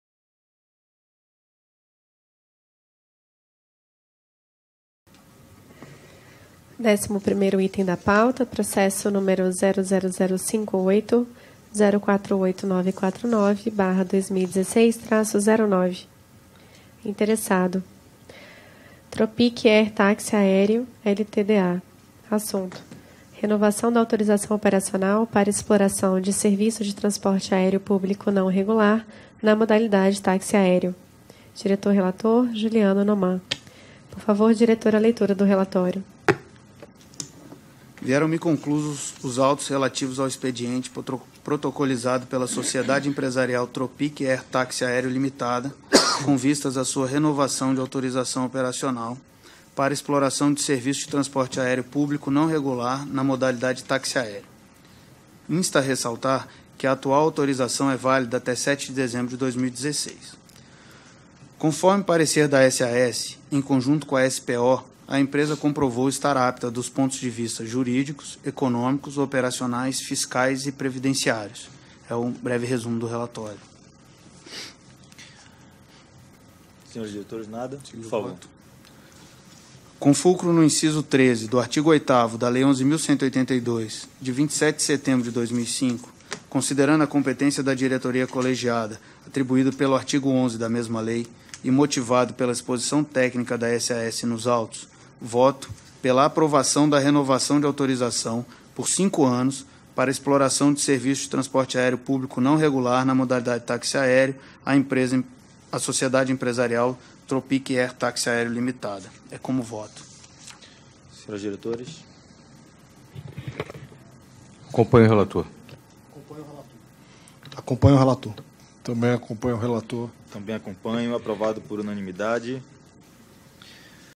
20ª Reunião Deliberativa da Diretoria - 2016
Assunto 11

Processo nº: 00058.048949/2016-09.

Interessado: Tropic Air Táxi Aéreo Ltda.

Assunto: Renovação da autorização operacional para exploração de serviço de transporte aéreo público não regular na modalidade táxi aéreo.